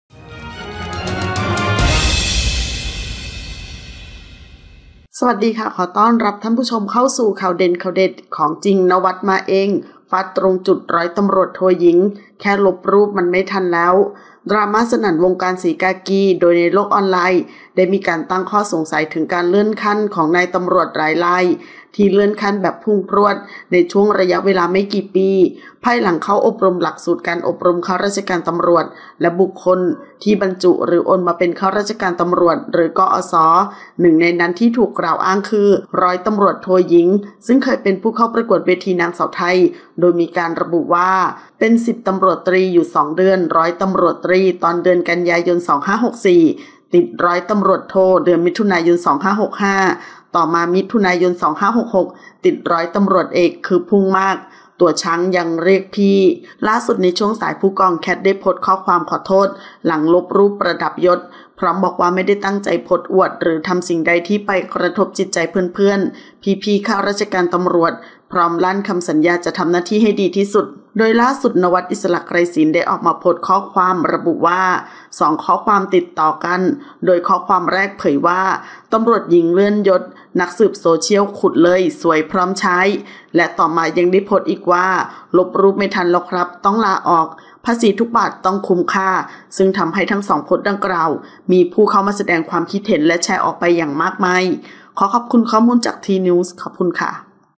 ของจริง! “ณวัฒน์” มาเอง ฟาดตรงจุด ร.ต.ท.หญิง แค่ลบรูปมันไม่ทันแล้ว
00:00 - Intro
00:10 - News
01:00 - Minute